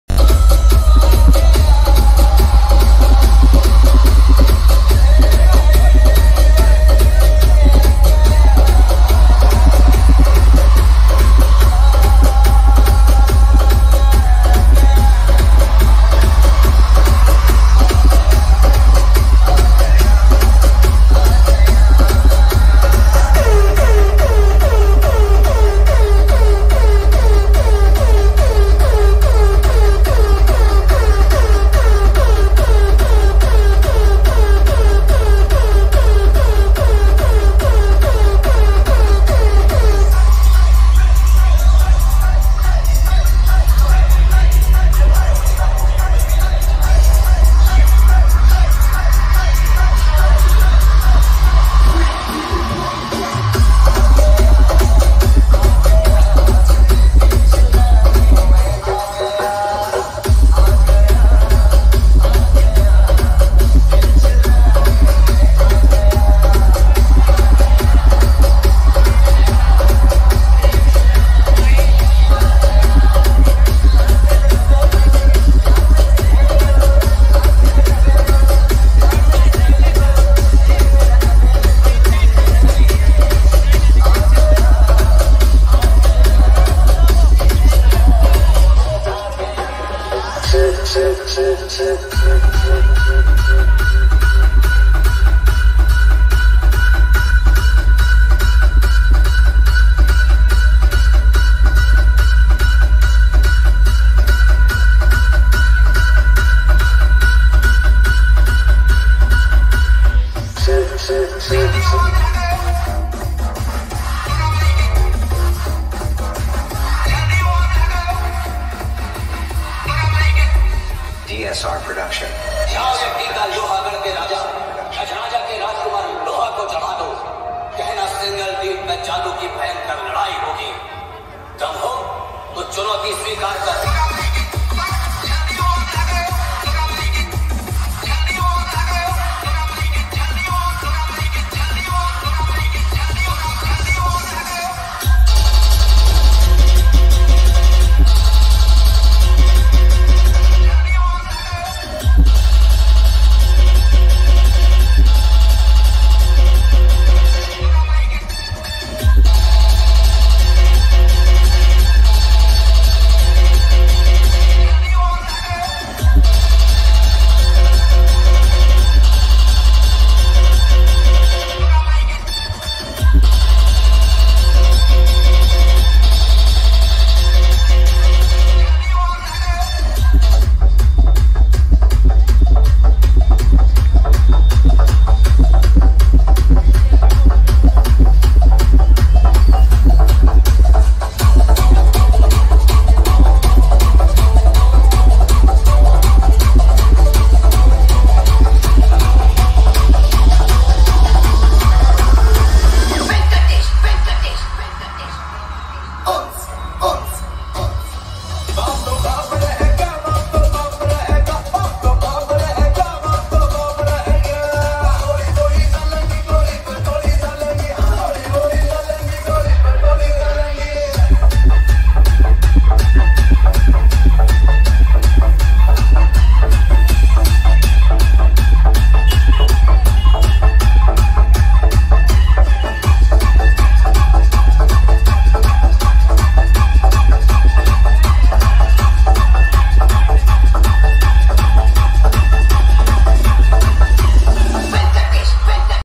ଶହଶହ ଲୋକଙ୍କ ମଝିରେ ନିଜର ଦମ୍ ଦେଖାଇଲା !! Dj Kb Pro Sound Yesterday Night Bhasani Program 2024 | By OPS
Visit Again.

🙏 Jay Maa Narayani 🙏